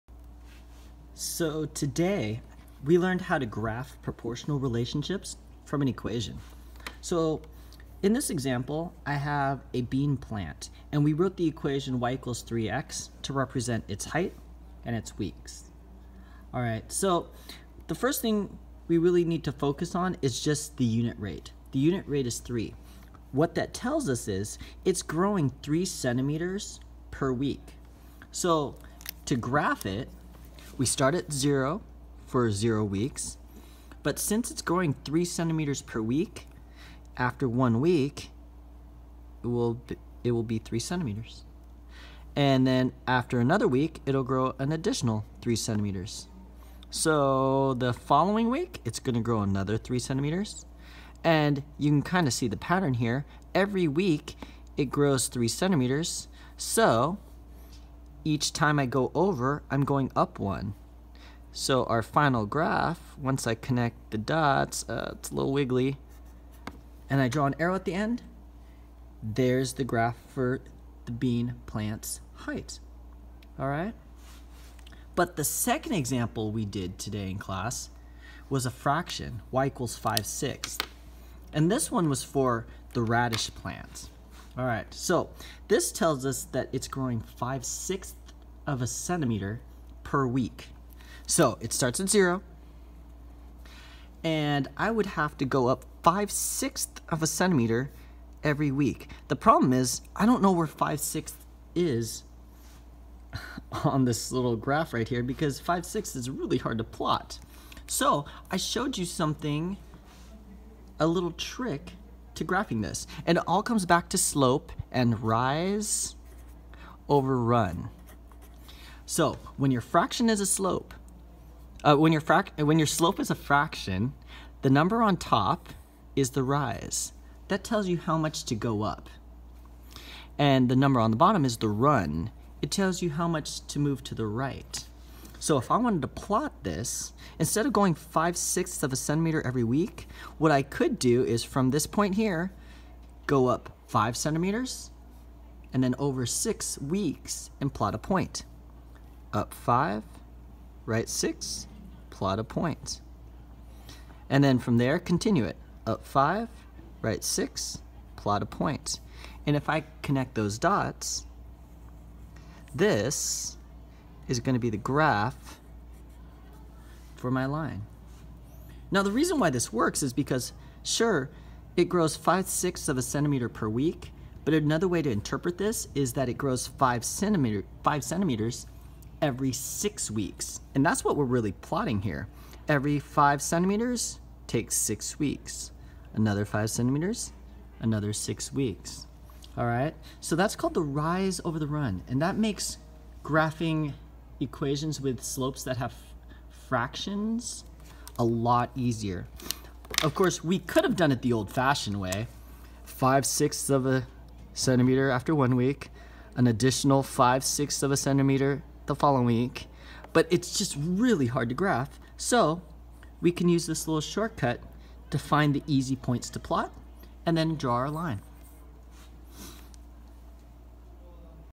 Graphing Proportional Relationships
Graphing can be confusing!  In this video, I show you how to graph a proportional relationship based on the rate of change (slope)!